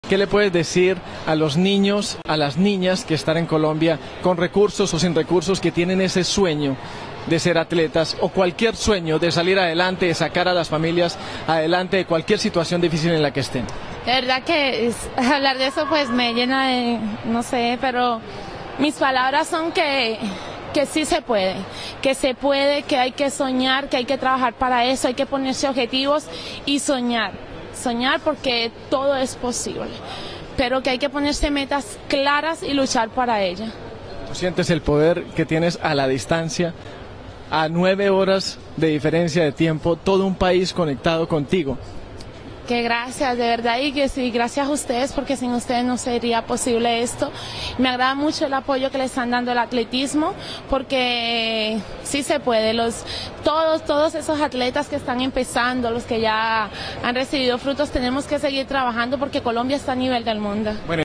Palabras conmovedoras de Caterine Ibargüen a todos los colombianos
campeona del mundo en Moscu 2013, mundial de Atletismo!! salto triple